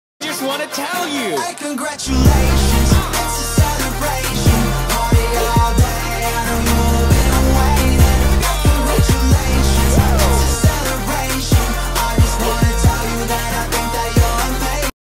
Congrats MrBeast
MrBeast hit 200 million today!